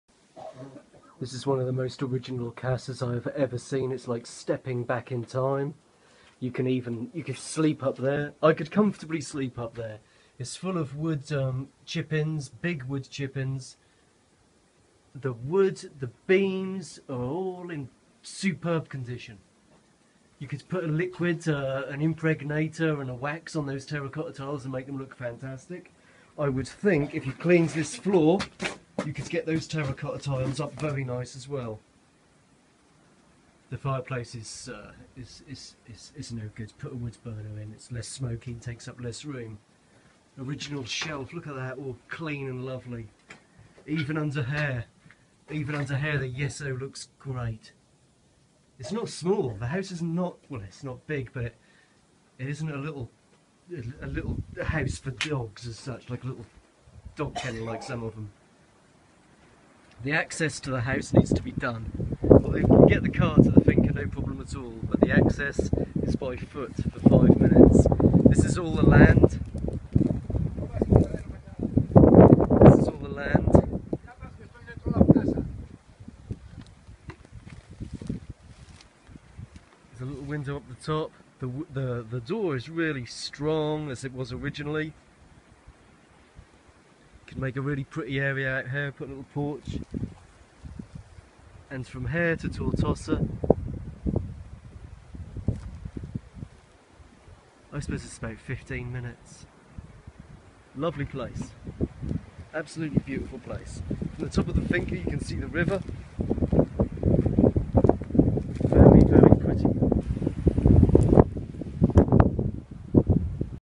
Finca Tivenys
Rural finca with stunning views in Tivenys catalunya